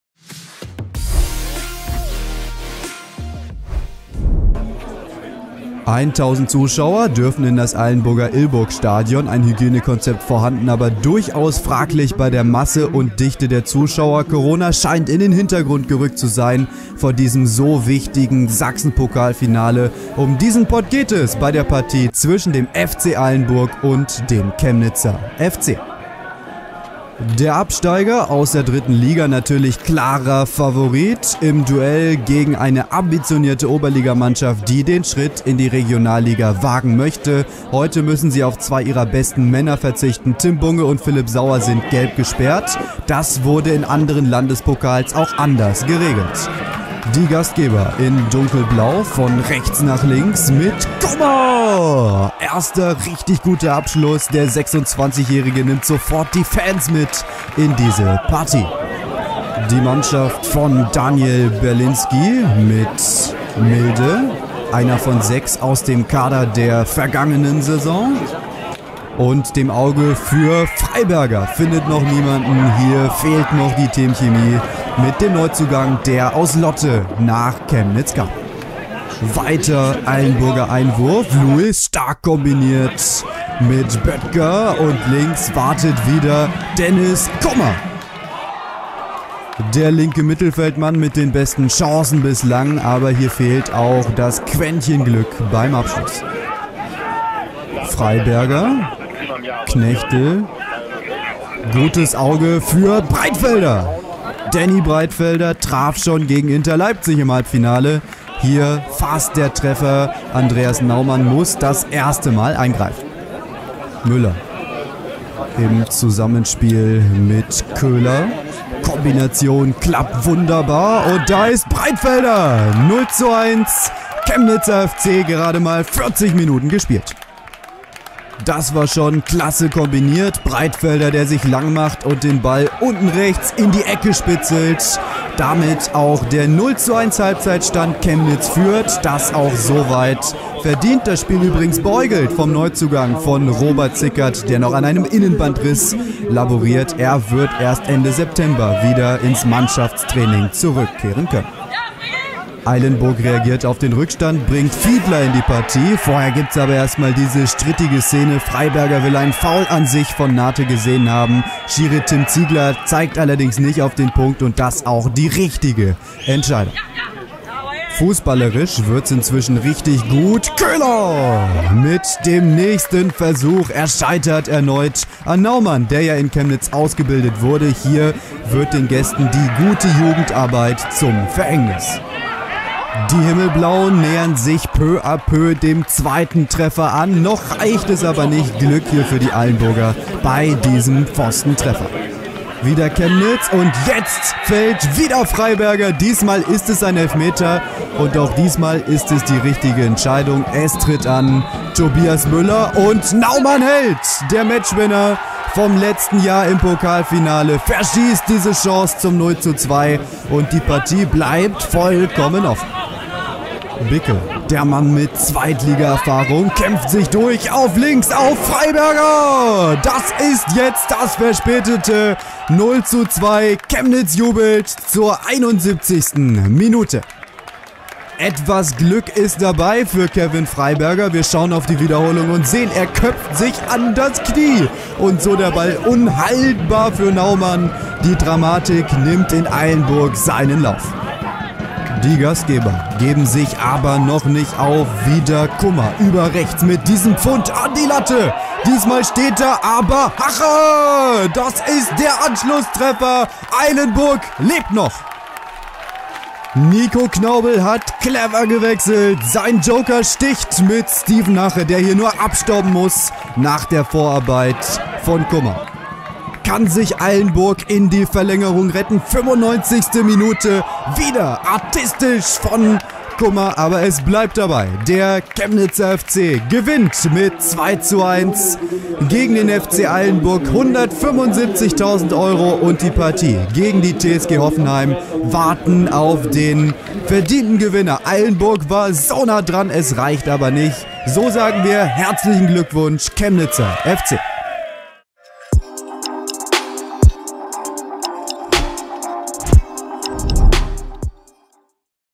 Nervenkitzel bis zum Schluss | FC Eilenburg – Chemnitzer FC (Sachsenpokal, Finale)
Schon im Halbfinale gelang dem FC Eilenburg eine Sensation, als man Lok Leipzig bezwingen konnte. Im Finale wartet nun mit dem Chemnitzer FC der nächste große Traditionsklub. Und wieder kämpfen die Eilenburger bis zum Schluss.

Finale (22.08.20)
Ergebnis: 1:2 (0:1)

Tore:
0:1 Danny Breitfelder
0:2 Kevin Freiberger
1:2 Steven Hache

Aufstellung FC Eilenburg:
1 Andreas Naumann, 6 Marko Trogrlic, 9 Dennis Kummer, 12 Marc Böttger, 13 Toni Majetschak, 15 Alec Nathe, 16 Alexander Vogel, 17 Christoph Bartlog, 19 Emilio Luis, 20 Lucas Danz, 25 Jonas Vetterlein

Einwechselspieler:
23 Emanuel Pickroth, 3 Hannes Pruchnik, 4 Steven Hache, 7 Sebastian Heidel. 10 Adam Fiedler, 11 Fabian Döbelt, 14 Jakob Funken

Trainer: Nico Knaubel

Aufstellung Chemnitzer FC:
1 Jakub Jakubov, 3 Niklas Hoheneder, 7 Christian Bickel, 9 Danny Breitfelder, 14 Tim Campulka, 16 Kevin Freiberger, 17 Lukas Knechtel, 19 Lukas Aigner, 23 Nils Köhler, 31 Paul Milde, 38 Tobias Müller

Einwechselspieler:
22 David Wunsch, 5 Jovan Vidovic, 6 Joannis Karsanidis, 8 Okan Adil Kurt, 13 Alexander Dartsch, 20 Roman Beko, 27 Theo Ogbidi

Trainer: Daniel Berlinski

Kommentar: Jan Ole Behrens
Produktion: Tobias Stedtler